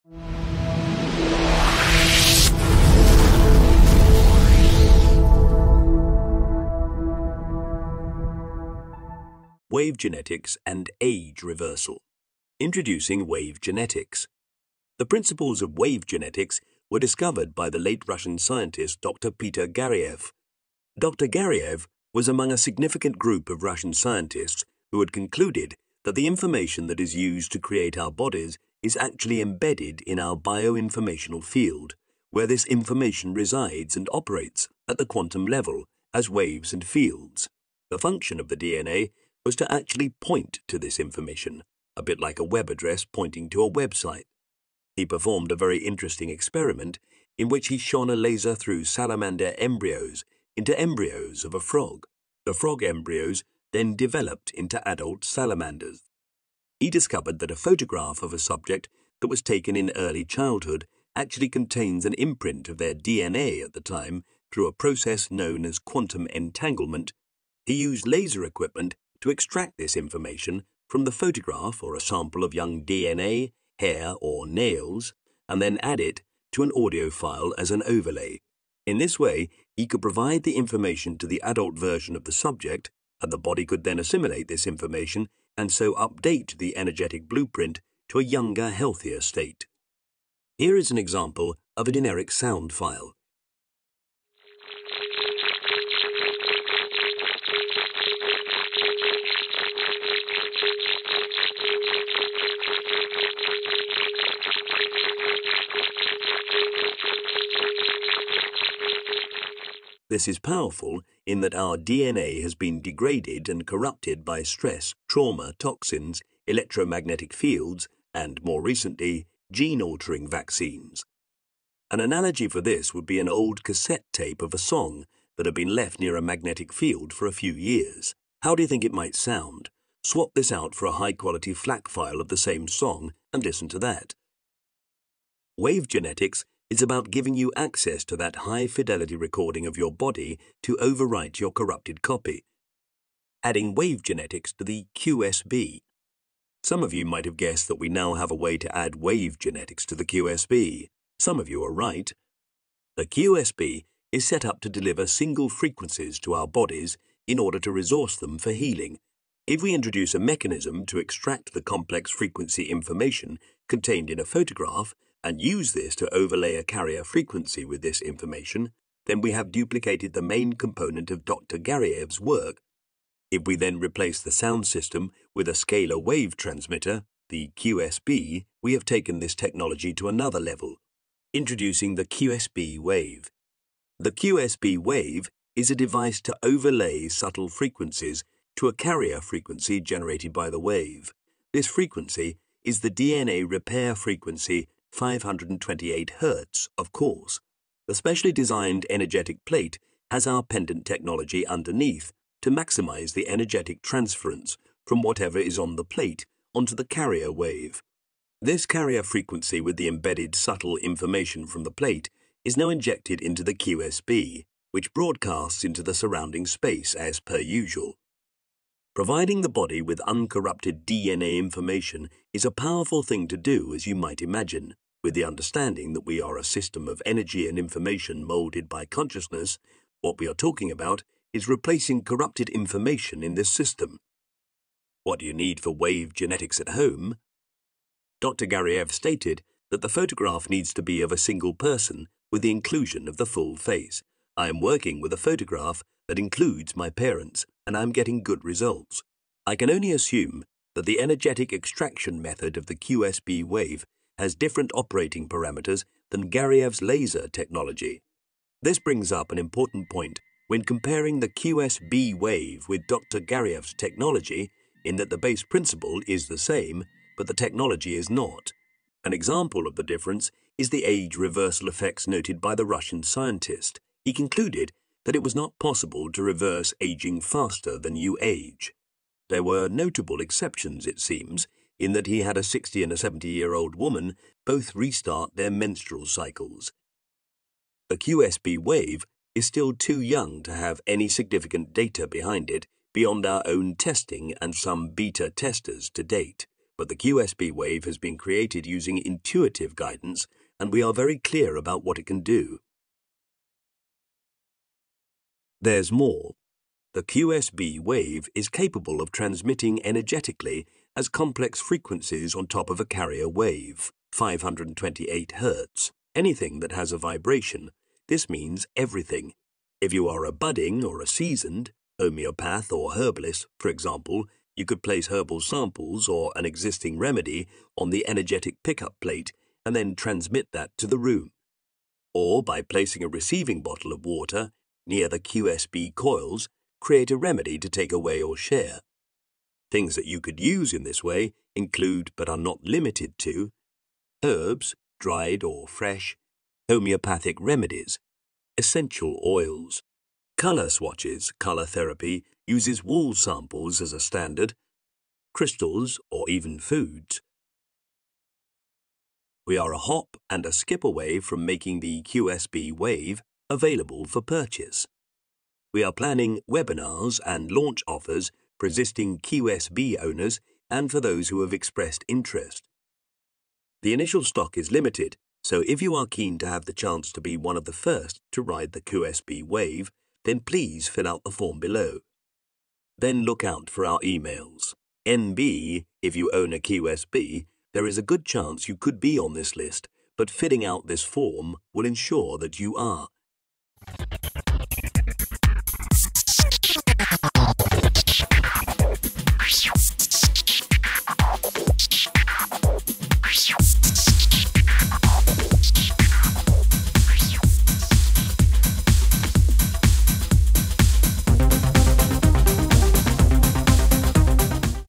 Wave Genetics and Age Reversal
The QSB Wave is a device that utilizes Wave Genetics principles to overlay subtle frequencies onto a carrier frequency for healing purposes. Wave Genetics involves extracting DNA information from photographs and incorporating it into an audio file, allowing the body to assimilate and update its DNA blueprint. By introducing this technology to the QSB (Quantum Scalar Box), it aims to provide uncorrupted DNA information for healing, repairing DNA, and potentially reversing the aging process - but using scalar waves instead of sound.